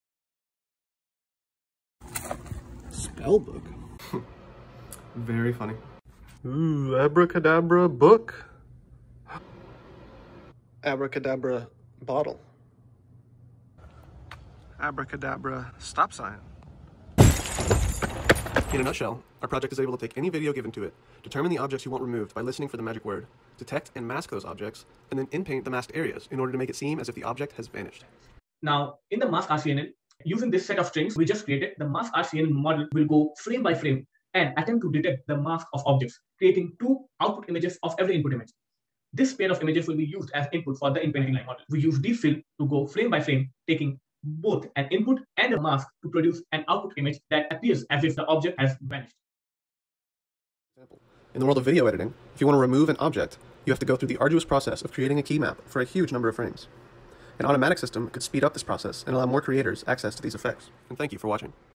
Demo CVPR 2023: Harry Potter's Vanishing Spell [Automatic Masking and Inpainting for Videos]
We created a project inspired by the Vanishing Spell from Harry Potter. Our project is a pipeline that seamlessly integrates the masking capabilities of MaskRCNN with the inpainting abilities of DeepFillv2 to remove up to 80 different types of objects from a video. All you have to do is say "Abracadabra [object]" in the video, and our system will automatically detect, mask, and inpaint the object, creating the illusion that it has disappeared.